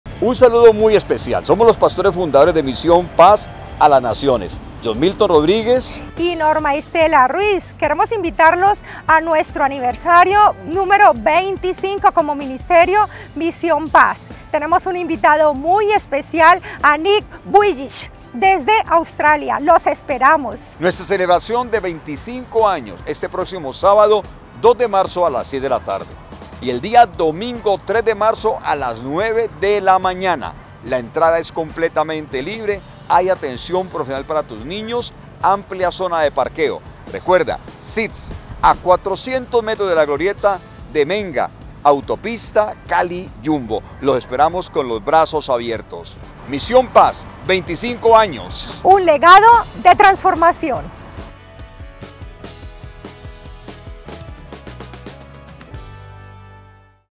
Gran Celebración, ya son 25 años - Misión Paz
Este año estamos de fiesta 🥳  Ya son 25 Años 🤩 y para darle inicio a nuestra celebración tenemos un gran invitado, @nickvujici desde Australia con su poderoso testimonio de lo que Jesús ha hecho en su vida. Este será un día de mucha bendición así que acompáñanos junto a toda tu familia, amigos y vecinos. Recuerda que la entrada es completamente gratis.
Te esperamos 👇
☑️Sábado 02 de Marzo 6:00 pm
☑️Domingo 03 de Marzo 9:00 am

📍 Calle 10 #36 -124 arroyohondo, autopista cali yumbo

👉🏻ENTRADA LIBRE -Atención especial para niños - Amplia zona de parqueo (gratuito)